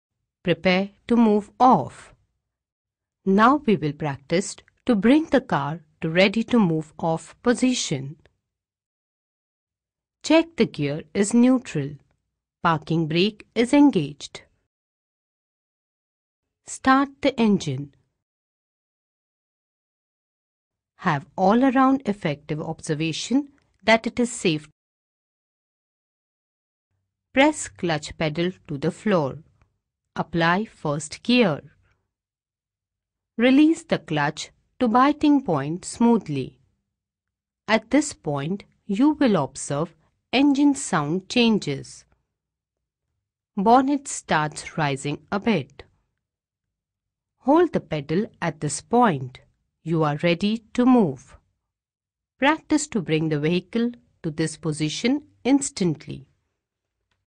Maruti Driving School MDS-IDTR Maruti Delhi tutorial Lessons | Prepare to Move on - Part 2
This is video is a share for fresh learner in India using a hatchback to learn how to start from scratch and learn how to drive a car (especially the females)

The Video was earlier fetched from the website of Maruti Driving School and is recorded at Gurugram by IDTR Delhi (Institutes Of Driving & Traffic Research are a joint venture between the Department of Transport of State Governments and Maruti Suzuki India Ltd. )